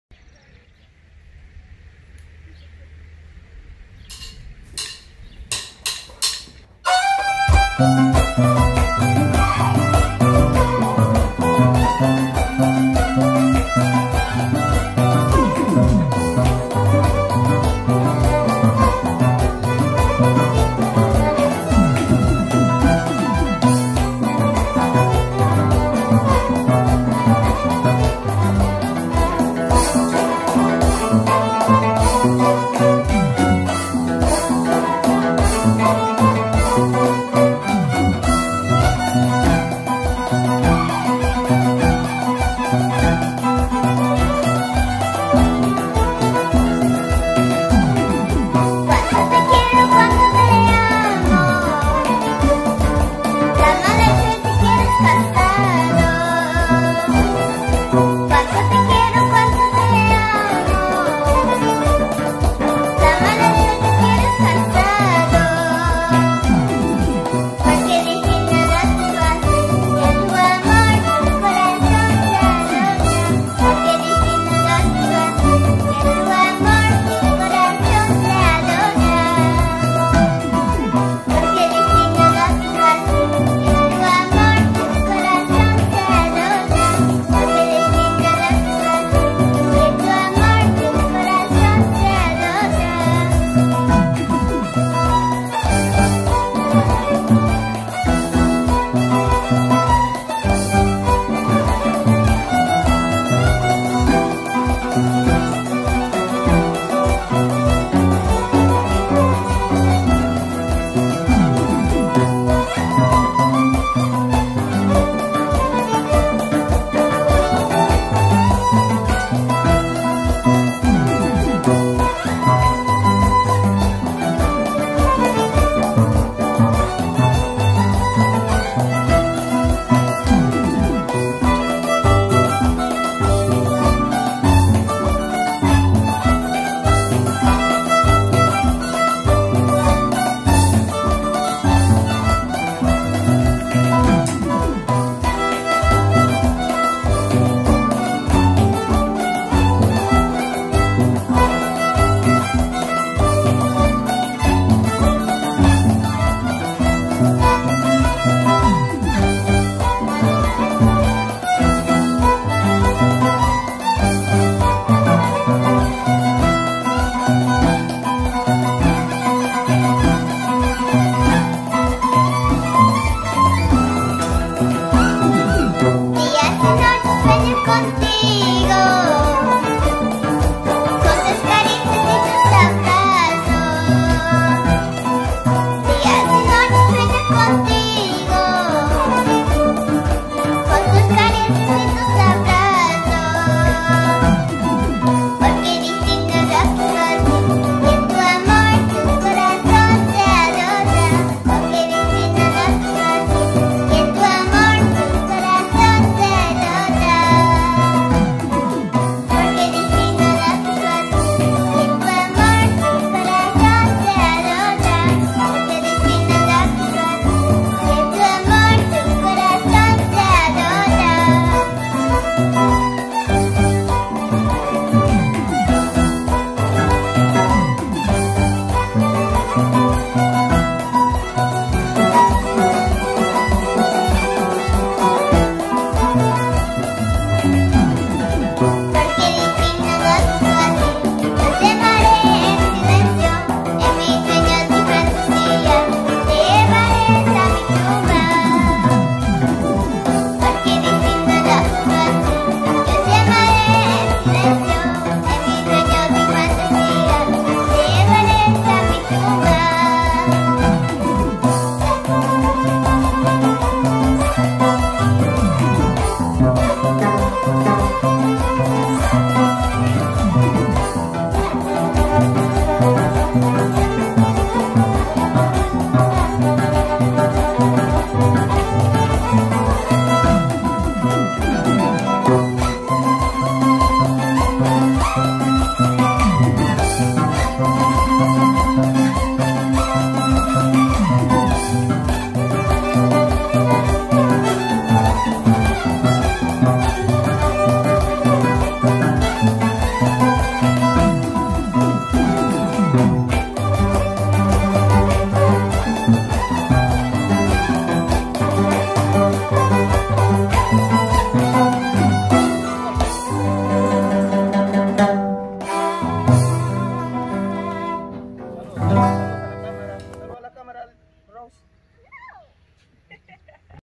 La Nieta de Los Hijos de Tintay - Rose Minaya en los ESTADOS UNIDOS
Video realizado en Estados Unidos una de las nietas de Santos Minaya - Rose Minaya